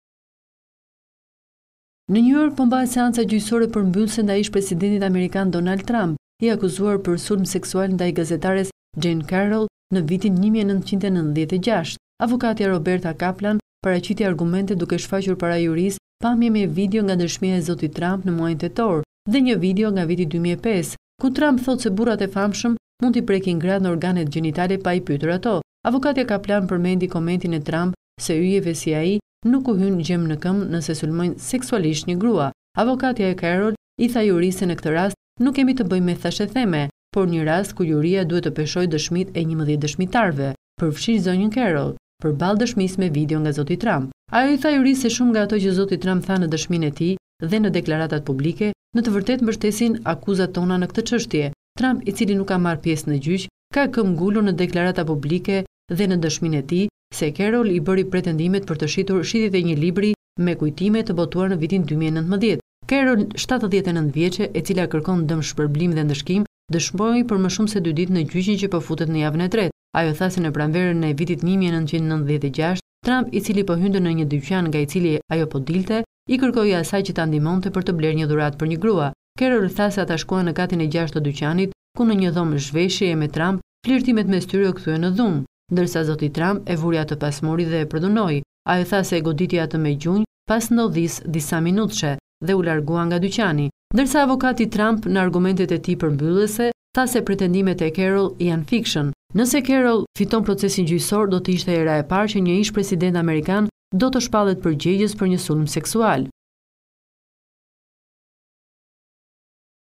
Mbahet seanca përmbyllëse, Trump akuzohet për përdhunim nga ish gazetarja Jean Carroll!
Është drejt përmbylljes procesi gjyqësor ndaj ish presidentit Dondal Trump, i akuzuar për sulm seksual nga ish gazetarja Jean Carroll, ngjarje e ndodhur në vitin 1996. Avokatja e Carroll, paraqiti para jurisë argumentet duke u bazuar në dëshmi e deklarata publike të bëra më herët nga trump. Ish presidenti nuk u paraqit në gjykatë, por avokati mbrojtës i quajti akuzat të stisura